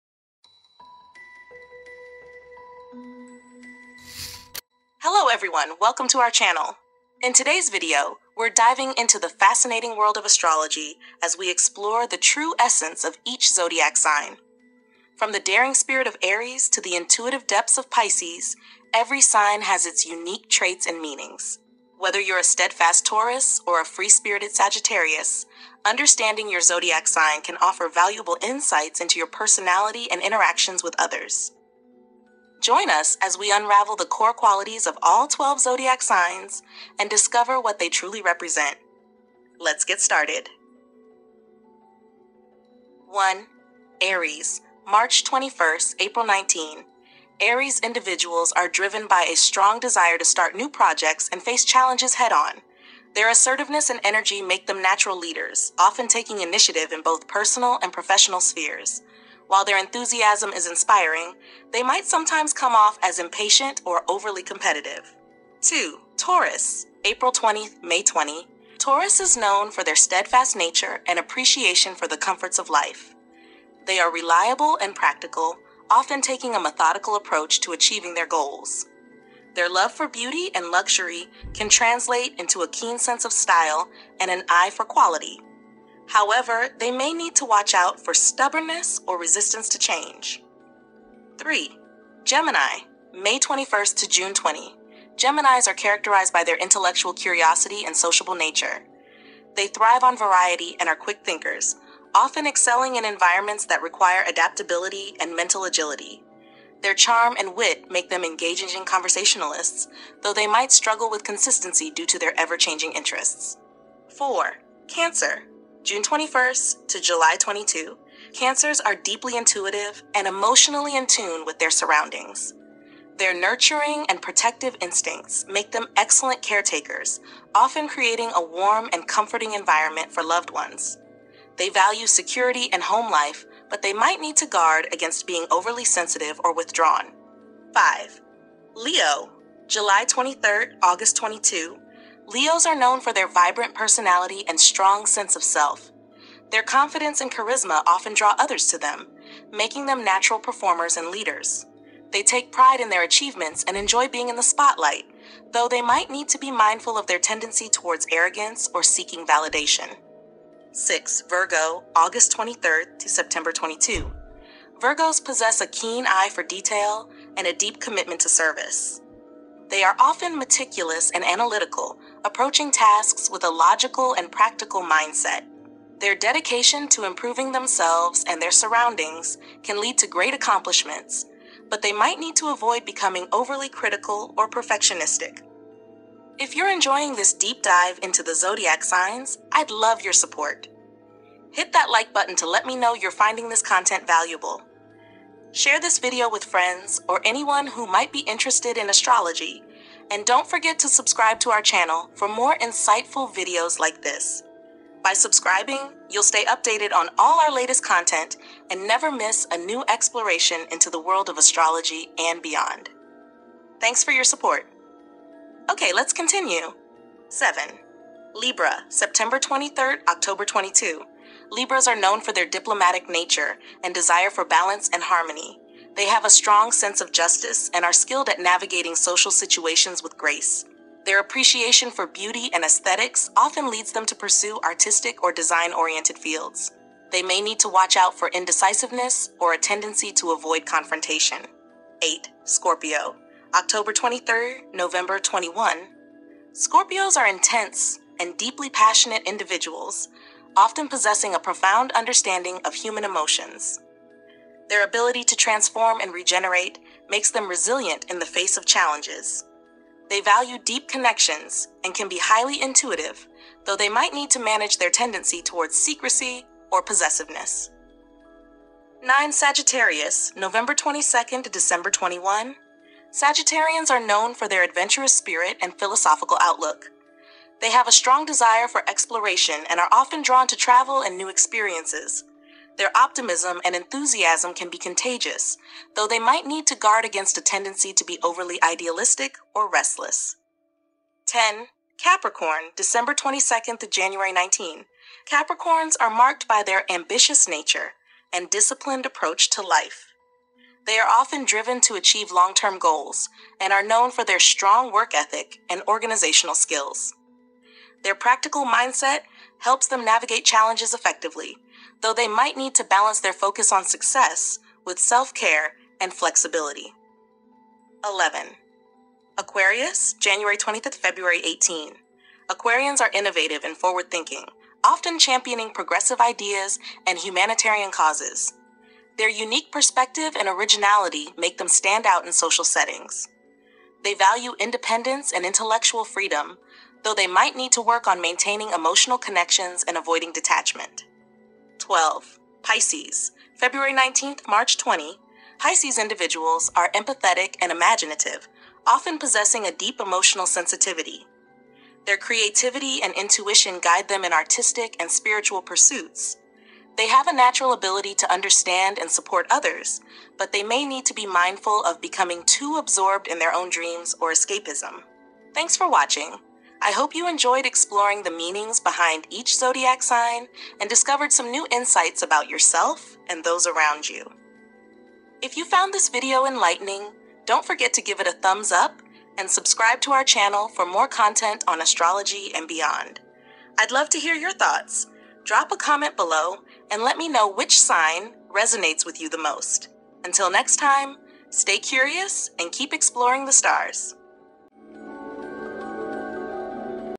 The 12 Zodiac Signs: What Each Sign Reveals About You
Curious about what your zodiac sign says about you? In this video, we explore the 12 zodiac signs and uncover the unique traits, strengths, and characteristics of each one. Whether you're an adventurous Aries, a practical Virgo, or a mysterious Scorpio, learn what your sign reveals about your personality, relationships, and more. Dive into the world of astrology and discover the deeper meaning behind your zodiac sign!

Chapters:
0:00 - Intro
01:00 - Main Video
06:00 - Outro